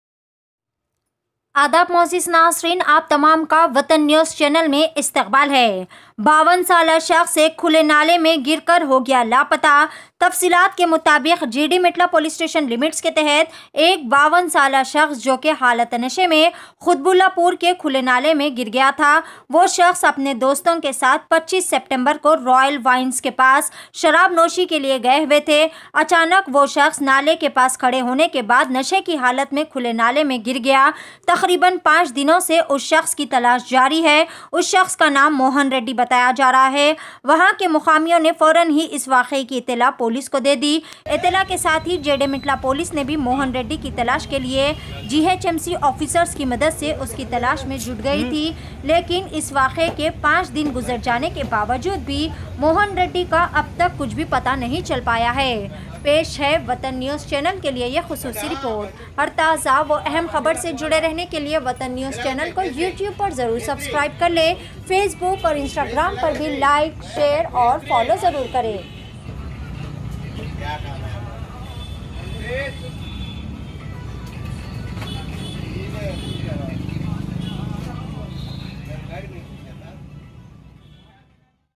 54-year-old man fell into an open Nala at Quthbullapur
Hyderabad: 54-year-old man fell into an open Nala at Quthbullapur.

He went out with friends for a drink. They went to Royal Wines in the evening. He went out to smoke and relieve himself. A missing complaint has been filed. Quthbullapur SHO K Balaraj told @NewsMeter_In